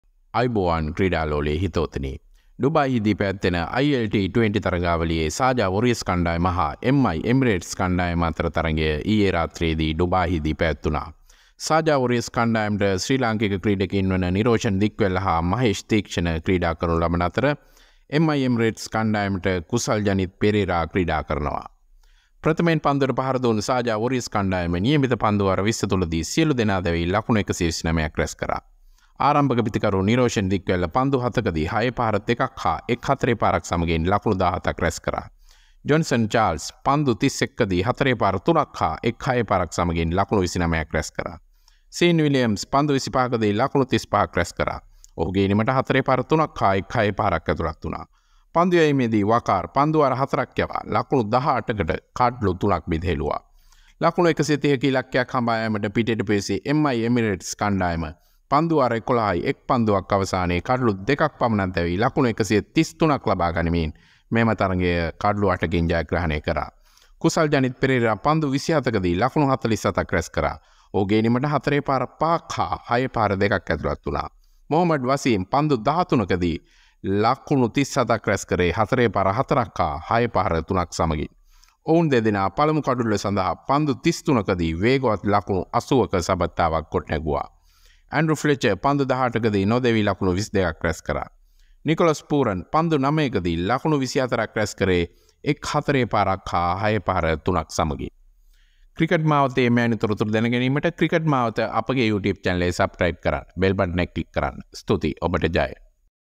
කුසල්ගේ වේගවත් ප්‍රහාර හමුවේ ඩුබායි ILT20 පිටිය කැළඹේ | MI Emirates Vs Sharjah Warriors ILT20 Match
MI Emirates Vs Sharjah Warriors ILT20 Match 18 Highlights | MIE Vs SW Highlights
MIE Vs SW INTERNATION LEAGUE T20 Highlights
Cricket Mawatha | Cricket News Sinhala